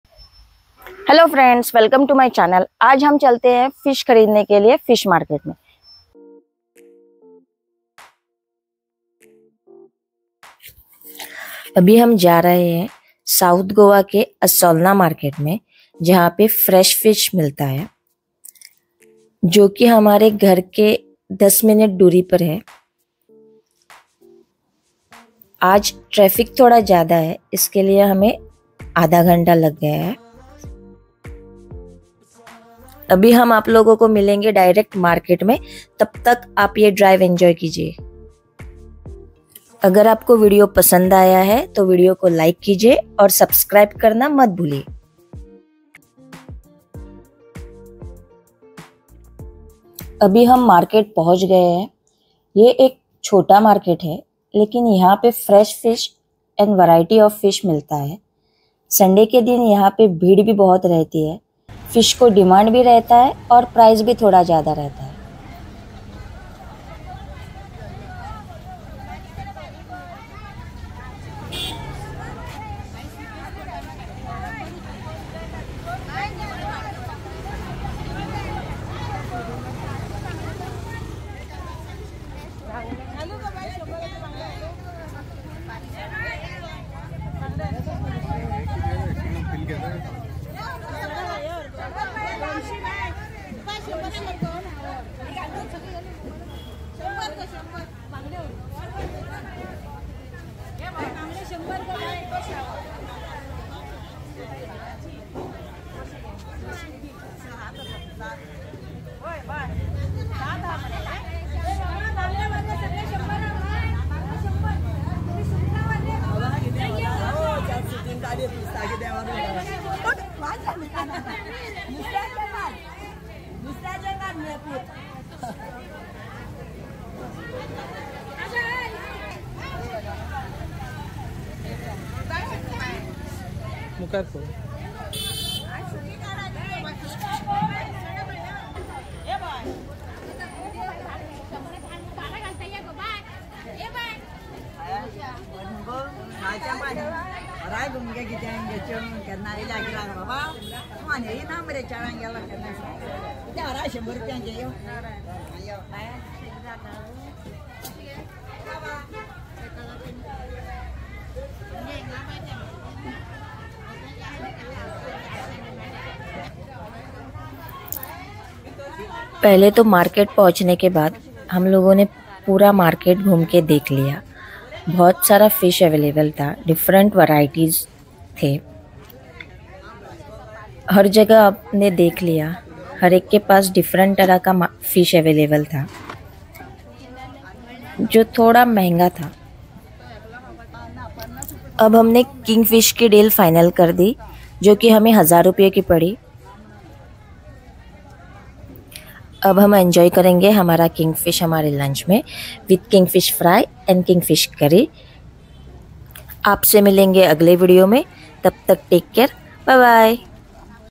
South Goa Fish Market | Assolna | Fresh Seafood | Fish Kingfish| Pomfret | Prawns | Goa Vlog 2023
Goa Vlog 2023 | Today we went to Assolna Fish market to buy fresh fish this is one of the best place to buy fresh fish in South Goa. Reasonable prices if you can bargain well.